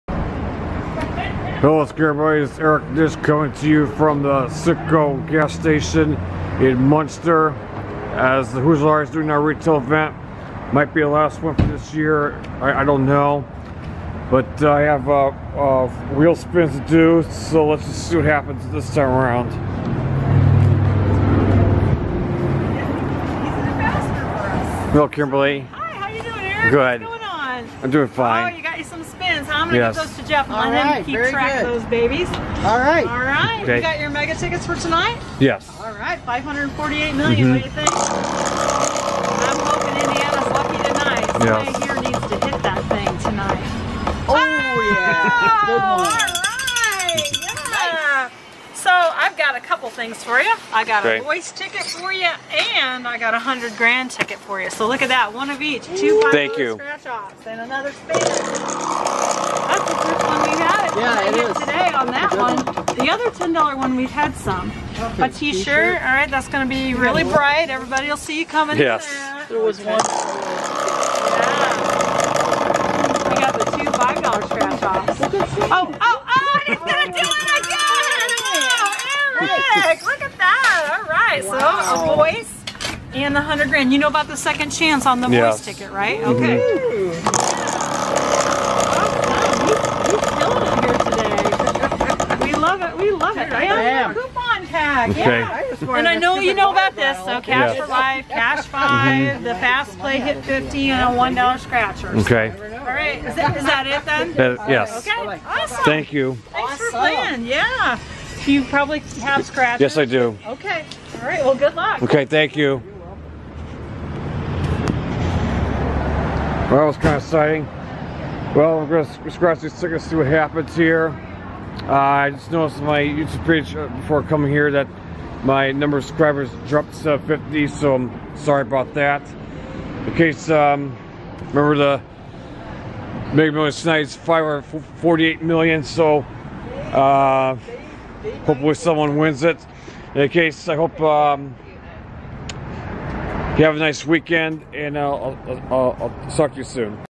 Sunday Special-Hoosier Lottery Retail Event Again
Here is me attending another retail event the Hoosier Lottery set up as they arrived at another Citgo station in Munster. What will I win on the wheel this time? Tune in to find out.